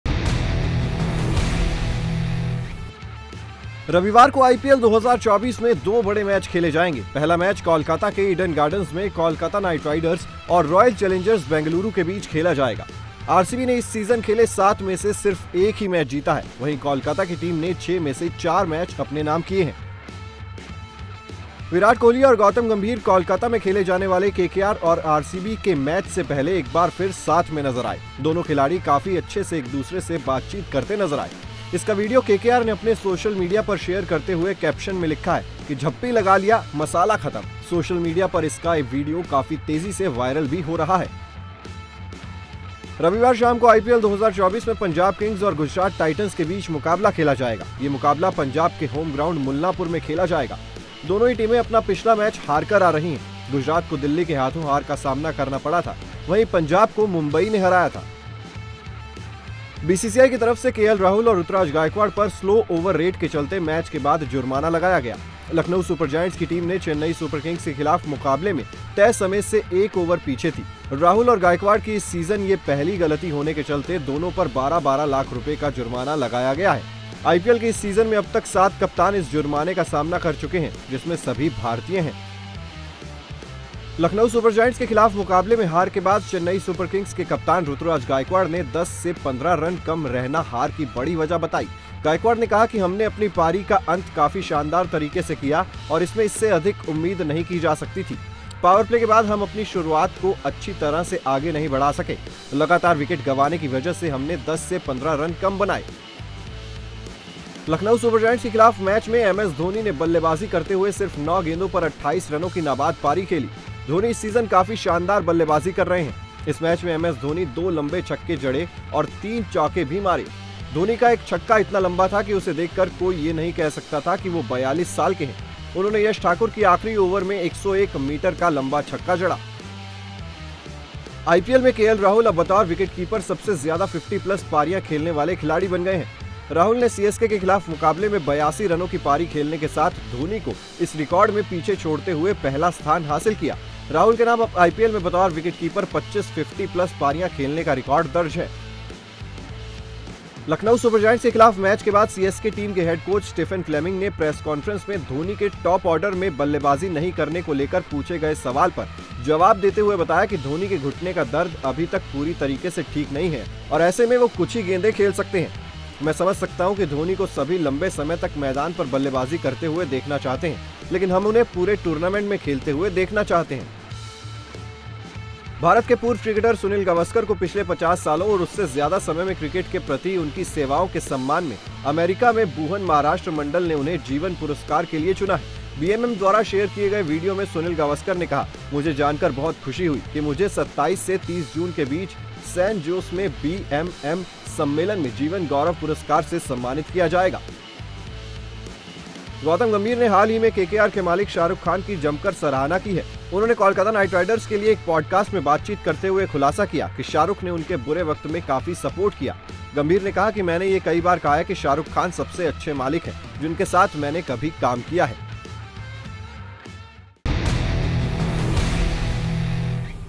Cricket Express: IPL 2024 में आज डबल हेडर, KKR और RCB होंगे आमने सामने, PBKS vs GT की टक्कर
रविवार को IPL 2024 में दो बड़े मैच खेले जाएंगे. पहला मैच Kolkata के ईडन गार्डन में KKR और RCB के बीच खेला जाएगा. RCB ने इस सीजन खेले 7 में से सिर्फ एक ही मैच जीता है.